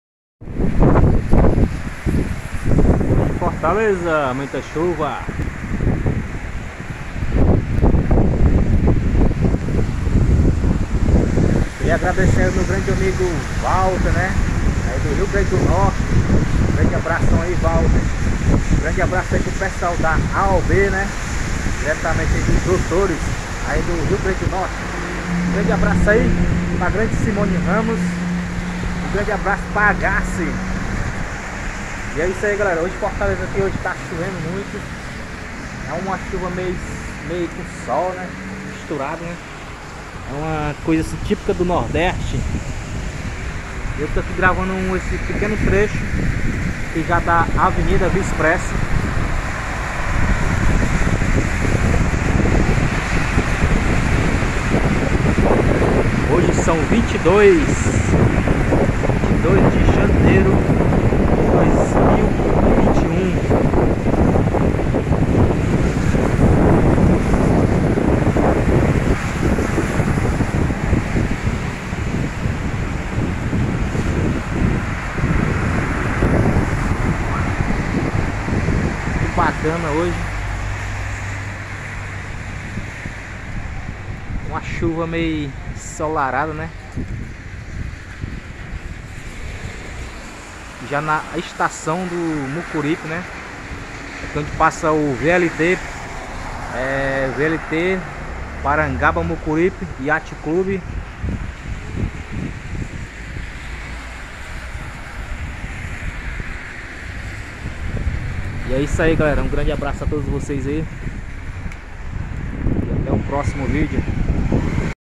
Av.engenheiro Alberto Sá Papicu Fortaleza Ceará.
Neste conteúdo de hoje mostro Av.engenheiro Alberto Sá Mais Conhecida como via Expressa papicu Fortaleza.